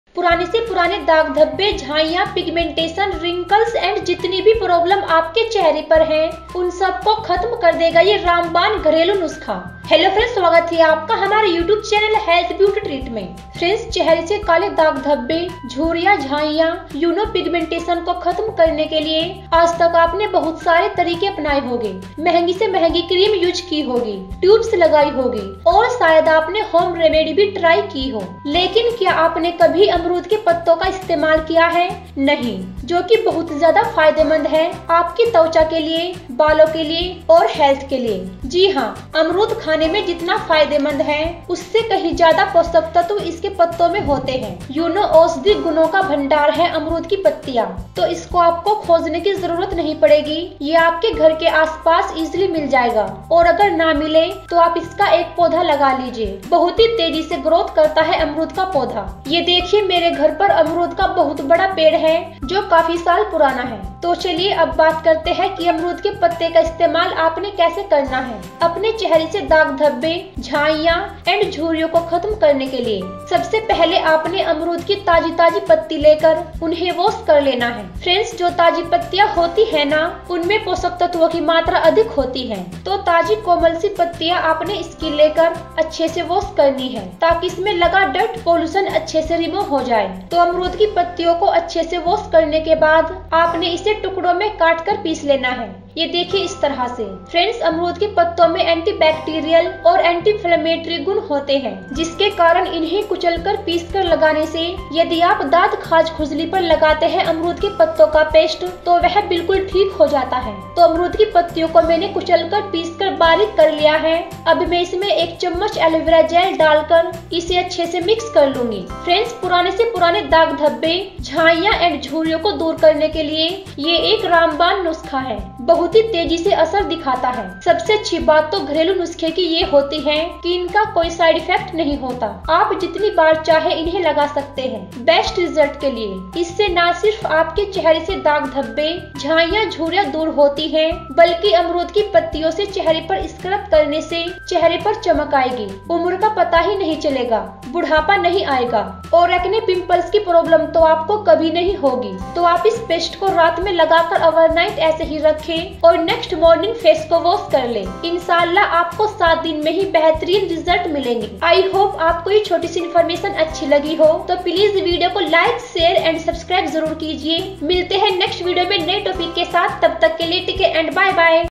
इसकी दो पत्तियां 7 दिन में चेहरे के पुराने से पुराने दाग-धब्बे, झाइयां,झुर्रियां सब खत्म कर देगी
इसकी दो पत्तियां 7 दिन में चेहरे के पुराने से पुराने दाग-धब्बे, झाइयां,झुर्रियां सब खत्म कर देगी, Home Remedies To Remove Dark Spots and Pigmentation on Face In 7 Days